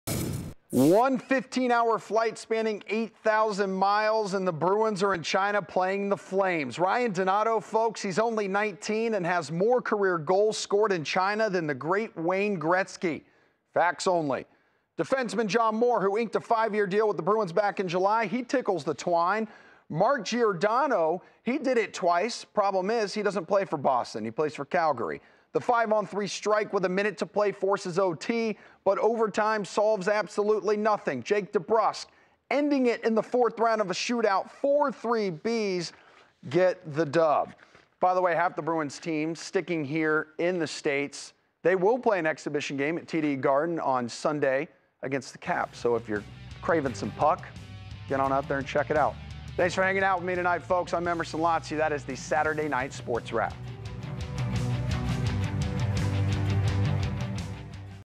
Bruins top Flames in preseason opener in China
Brad Marchand and Jake DeBrusk scored in the shootout to lead the Boston Bruins to a 4-3 win over the Calgary Flames in the NHL exhibition season opener on Saturday in Shenzhen, China.

Highlights are from Emerson Lotzia's Boston 25 sportscast on September 15th.